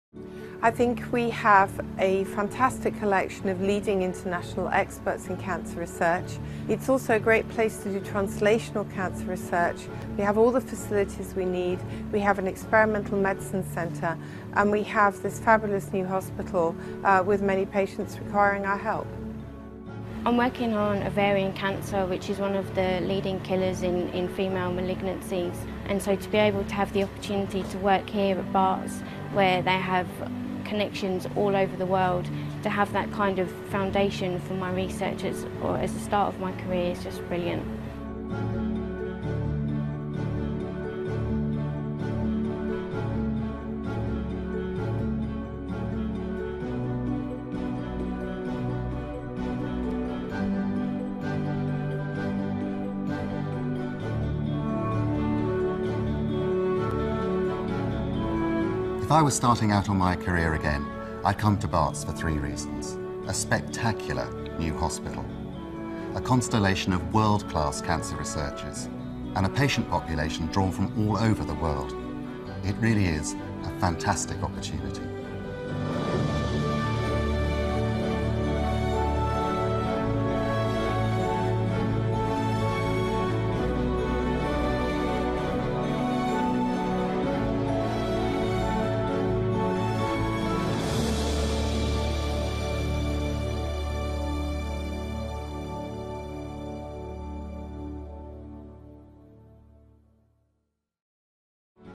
Barts Cancer Institute (BCI) Opportunities
Barts Cancer Institute is an excellent place to pursue a career in cancer medicine and clinical research, right in the heart of London.